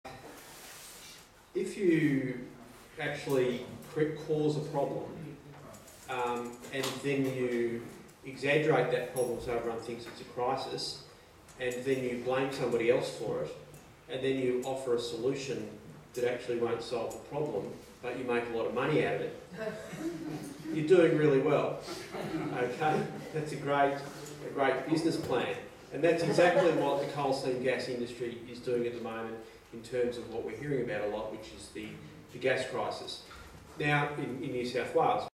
Excerpt #1
An excerpt from a talk given by Mark Ogge at a special meeting of Stop CSG Illawarra on September 15th.